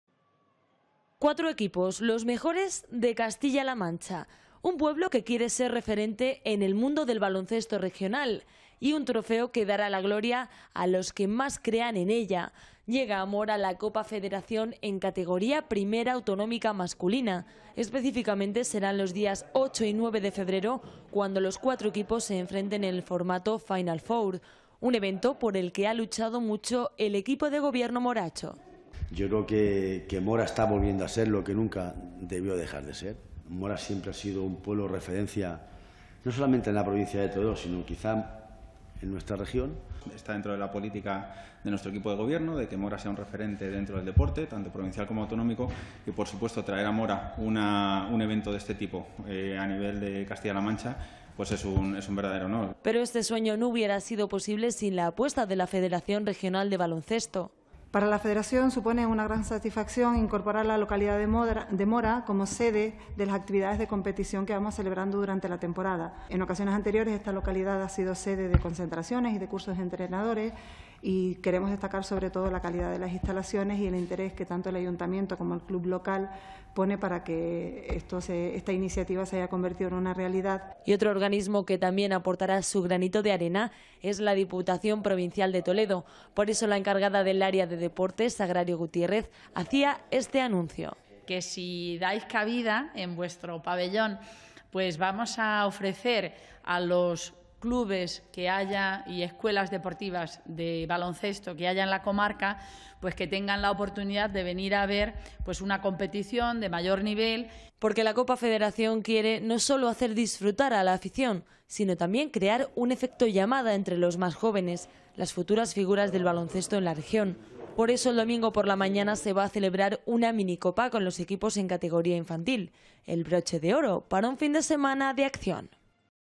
La Copa Federación llega a Mora (Toledo)
Este fin de semana se disputará en Mora la Copa Federación de Categoría 1ª Autonómica Masculina. Serán los cuatro mejores equipos los que luchen por ella: Mora, Miguel Esteban, Campo de Criptana y Talavera. Teletoledo Noticias.